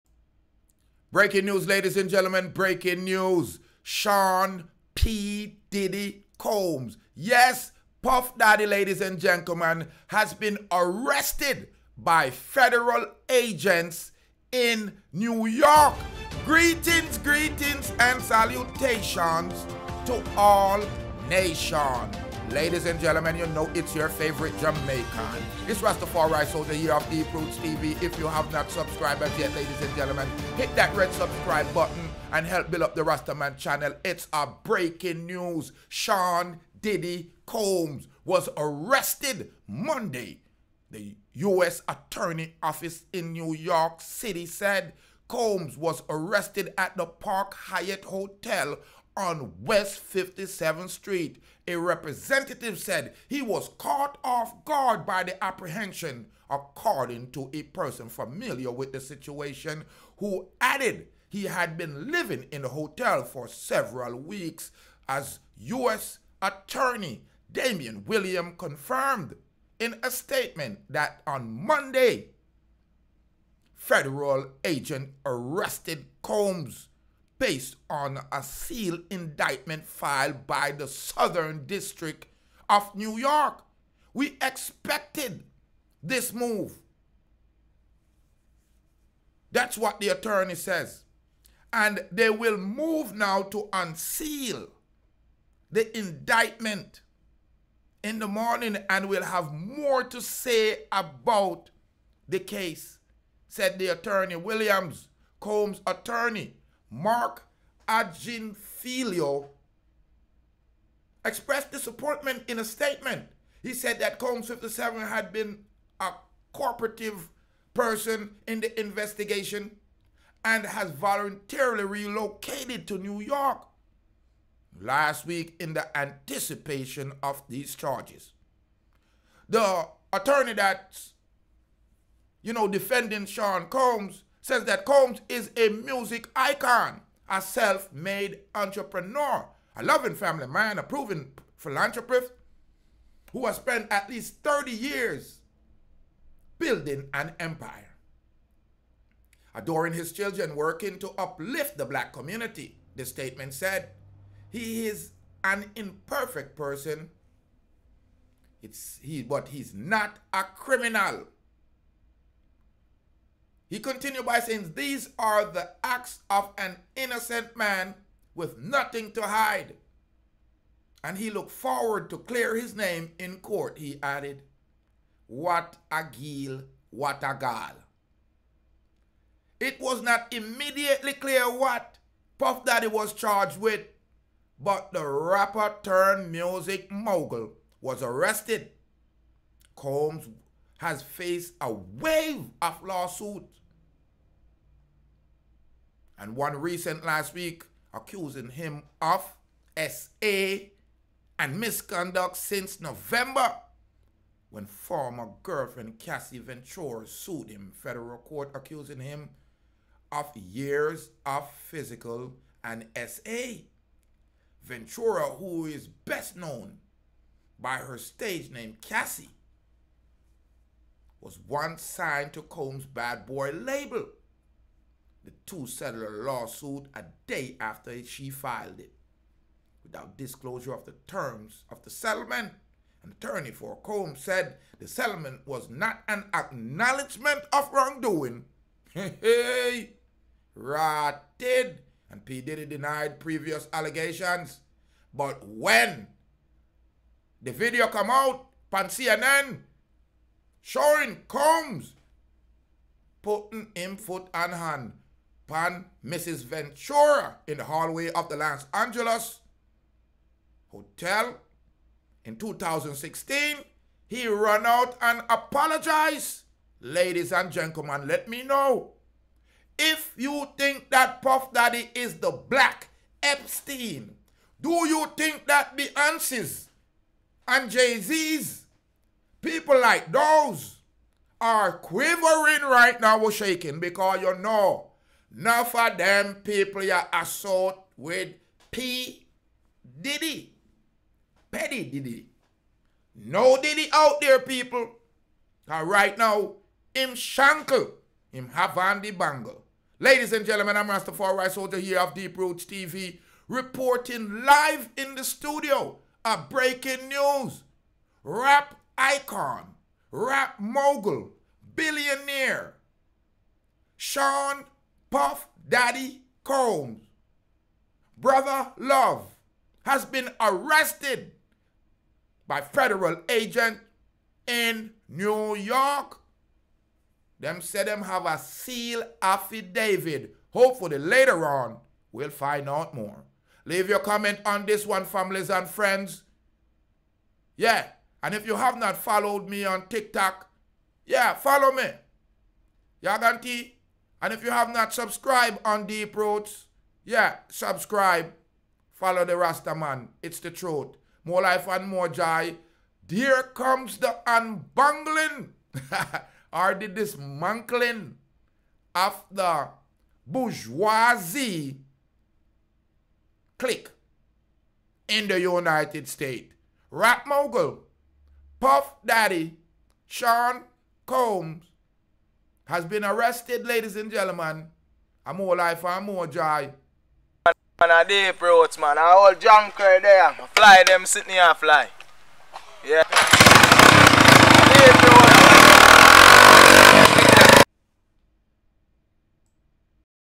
Sean “Diddy” Combs arrested News
Sean 'Diddy' Combs arrested by federal agents in New York
Sean “Diddy” Combs was arrested Monday, the U.S. Attorney’s Office in New York City said.

Combs was arrested at the Park Hyatt hotel on West 57th Street, a representative said. He was caught off-guard by the apprehension, according to a person familiar with the situation, who added he had been living at the hotel for several weeks.

U.S. Attorney Damian Williams confirmed in a statement that on Monday evening, federal agents arrested Combs based on a sealed indictment filed by the Southern District of New York.

“We expect to move to unseal the indictment in the morning and will have more to say at that time,” Williams said.